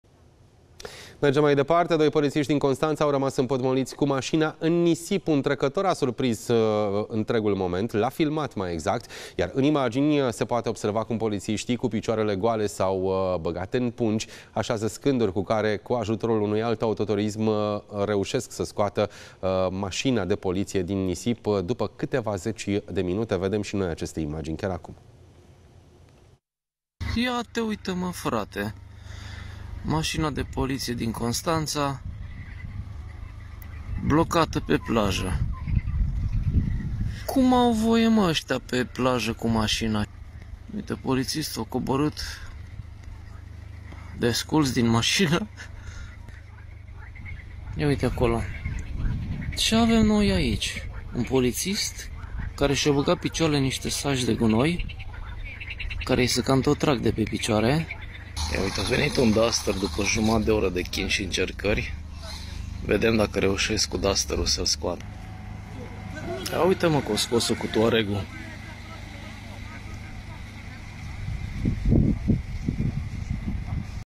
Polițiști la apă: au rămas cu mașina împotmolită pe plajă
Doi polițiști din Constanța au rămas împotmoliți cu mașina în nisip. Un trecător a surprins întregul moment, iar în imagini se poate observa cum polițiștii, cu picioarele goale, s-au băgat în pungi, așază scânduri și cu ajutorul unui alt autoturism reușesc să scoată mașina din nisip, după câteva zeci de minute.